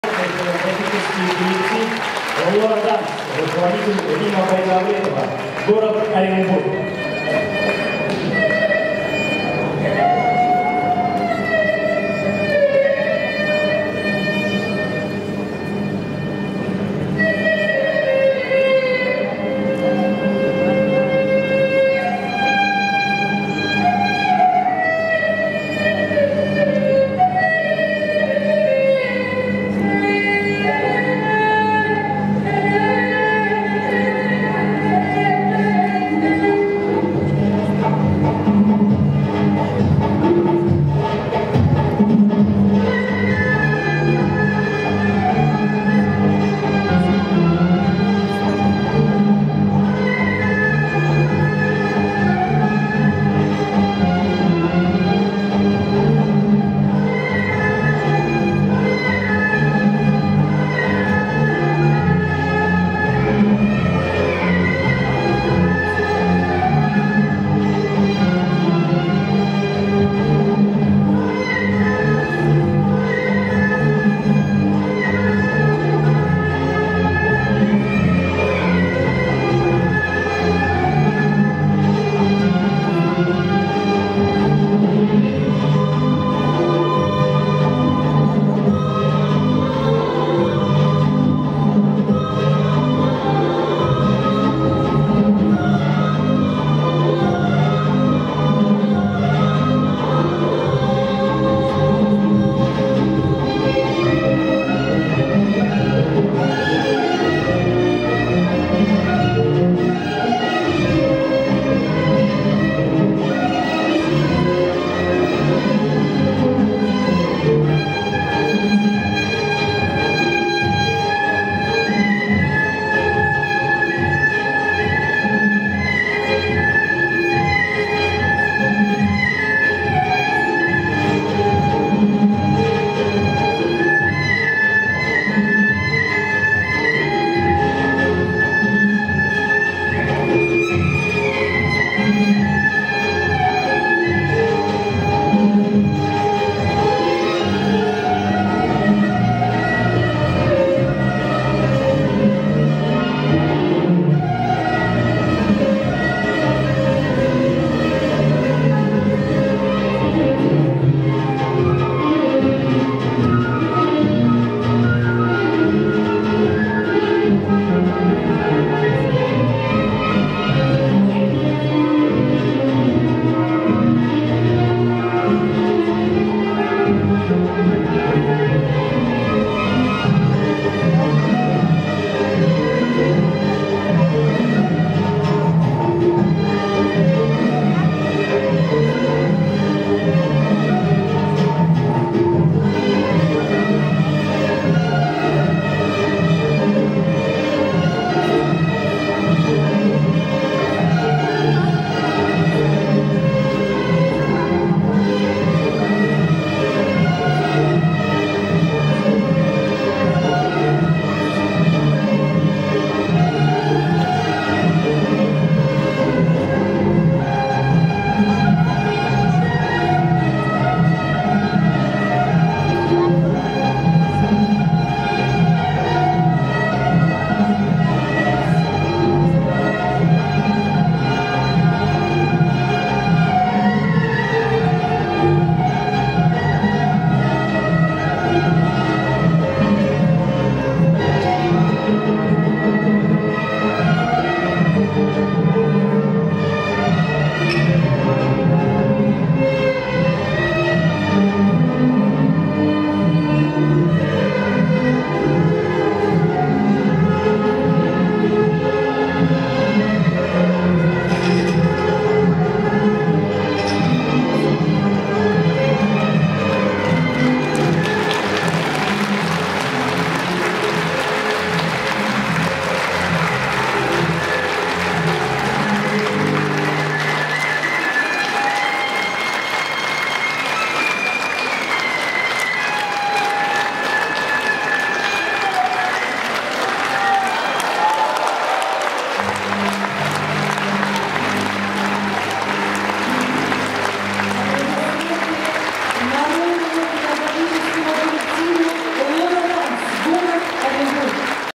Народный хореографический коллектив "Лора-данс"
Руководитель Рима Байдавлетова
МБУ ДК "Молодёжный" г.Оренбург
Гала концерт "Обильный край, благословенный!" 2019